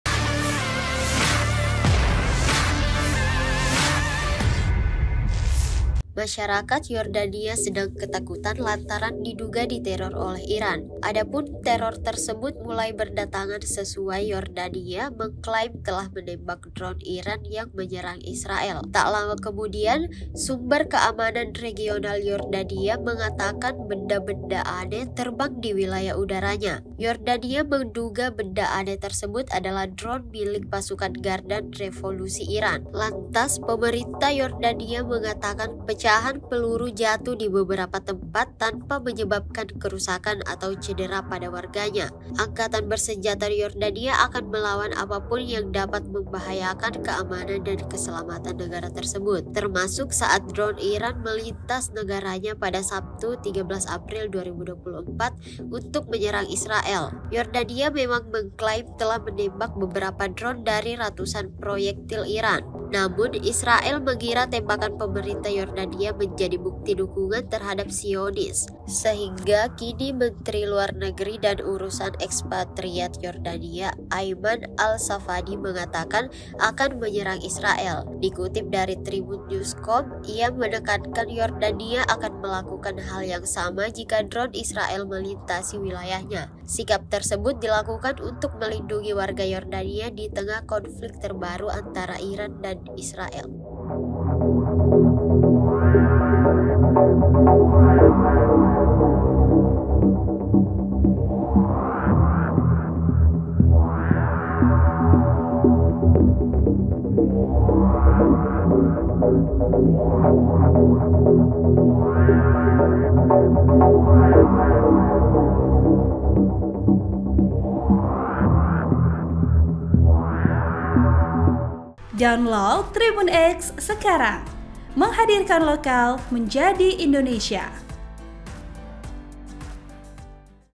Masyarakat Yordania Ketar-ketir Diteror Iran Imbas Bantu Israel!
TRIBUN-TIMUR.COM - Masyarakat Yordania sedang ketakutan lantaran diduga diteror oleh Iran.

Adapun teror tersebut mulai berdatangan seusai Yordania mengklaim telah menembak drone Iran yang menyerang Israel.

Tak lama kemudian, sumber keamanan regional Yordania mengatakan benda-benda aneh terbang di wilayah udaranya.

Yordania menduga benda aneh tersebut adalah drone milik pasukan garda revolusi Iran (IRGC).

Lantas, pemerintah Yordania mengatakan pecahan peluru jatuh di beberapa tempat tanpa menyebabkan kerusakan atau cedera pada warganya.

“Beberapa pecahan peluru jatuh di beberapa tempat pada saat itu tanpa menyebabkan kerusakan berarti atau cedera pada warga,” tambah media pemerintah Yordania dikutip dari Reuters.

Angkatan bersenjata Yordania akan melawan apapun yang dapat membahayakan keamanan dan keselamatan negara tersebut.

Termasuk saat drone Iran melintasi negaranya pada Sabtu (13/4) untuk menyerang Israel.

Yordania memang mengklaim telah menembak beberapa drone dari ratusan proyektil Iran.

Namun, Israel mengira tembakan pemerintah Yordania menjadi bukti dukungan terhadap Zionis.

Sehingga kini, Menteri Luar Negeri dan Urusan Ekspatriat Yordania, Ayman Al-Safadi mengatakan akan menyerang Israel.

Dikutip dari Tribunnews.com, ia menekankan Yordania akan melakukan hal yang sama jika drone Israel melintasi wilayahnya.

Sikap tersebut dilakukan untuk melindungi warga Yordania di tengah konflik terbaru antara Iran dan Israel.

Naskah: Nurul Arifah S. Torro
VO: Andi Siti Nurhaliza S (Mahasiswa Magang UNM)
VP: Asrul

(TRIBUN-TIMUR.COM)